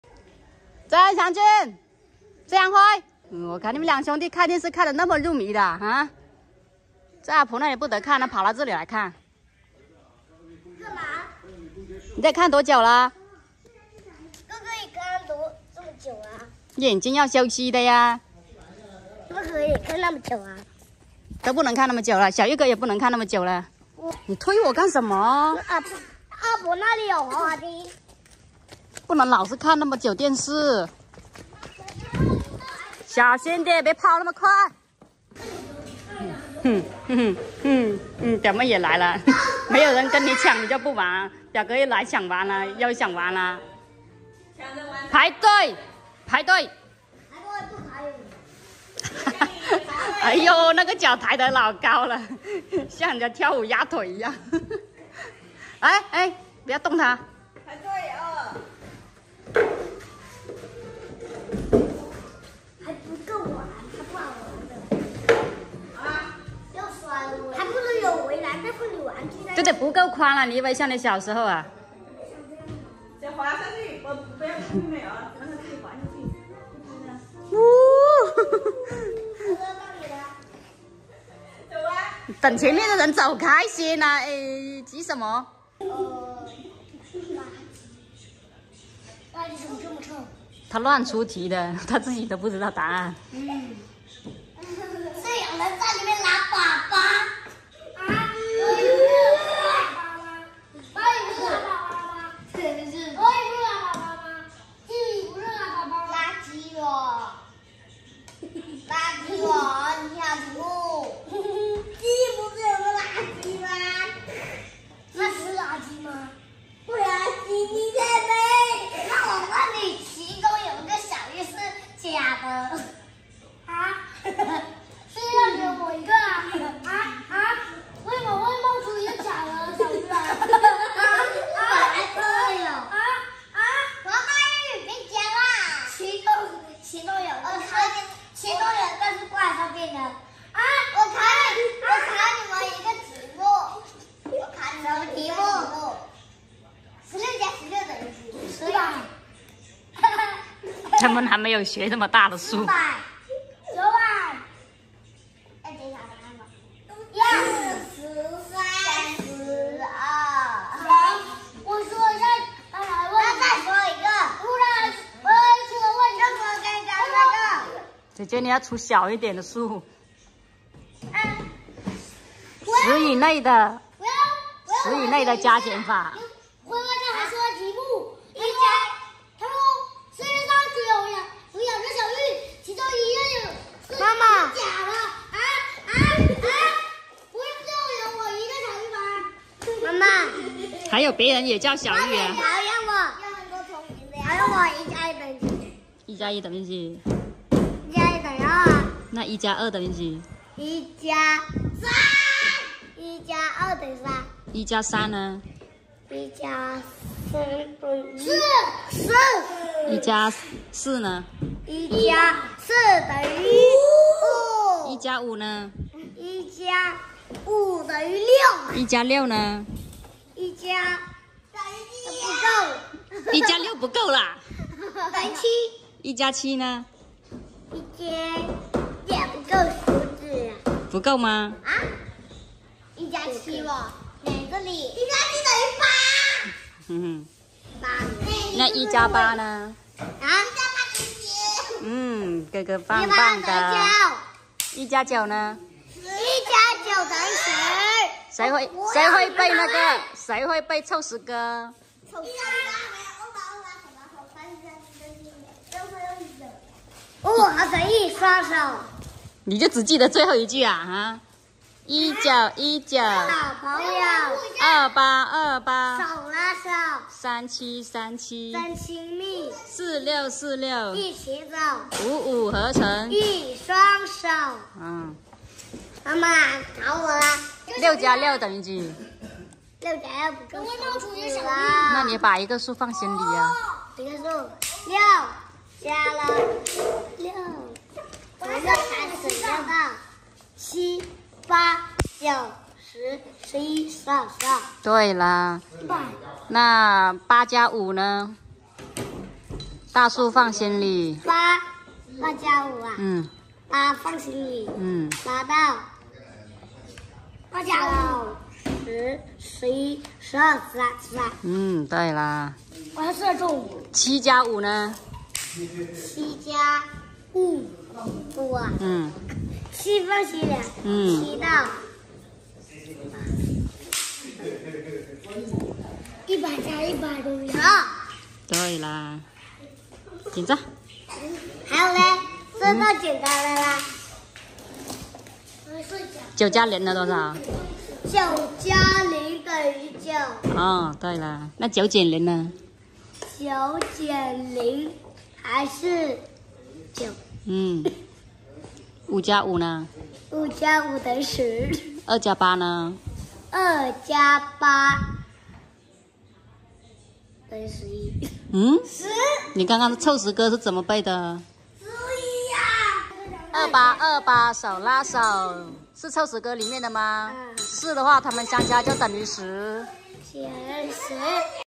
辉辉和姐姐互相出题考对方，童真的言语和欢笑让人感慨童年真好
记录农村宝宝成长生活，开心快乐与您分享！